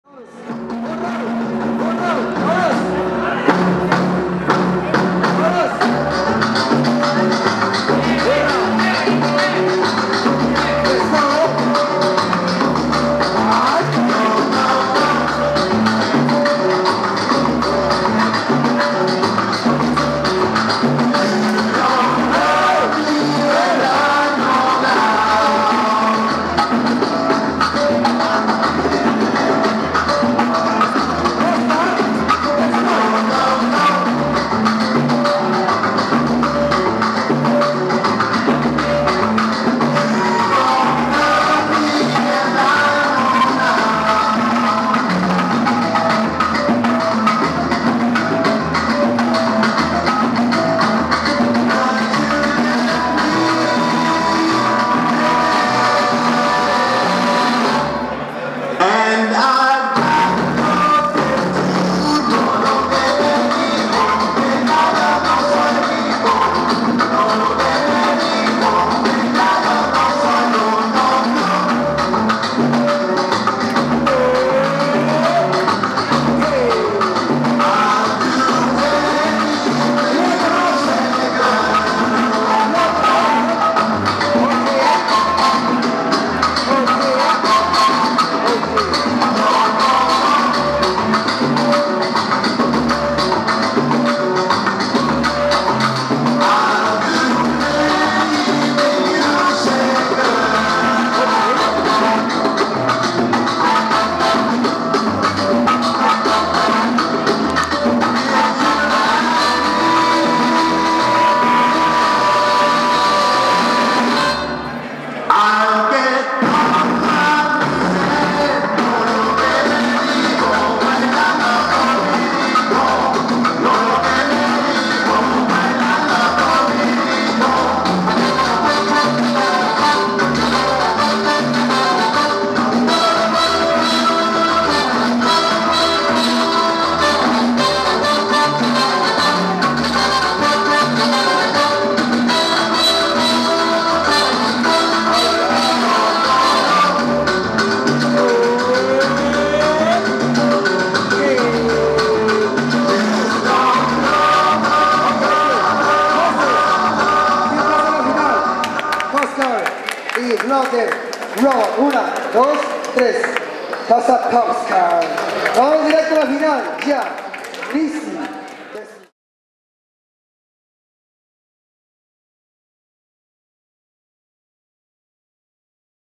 Popskar vs. Blop The Robot Allstyles SoloBaile SEMI FINAL | 2018 MEXICO CITY YAK
1 on 1 Allsyles SEMI FINAL of the Campeonato Solo Baile in Mexico City

Winner: Popskar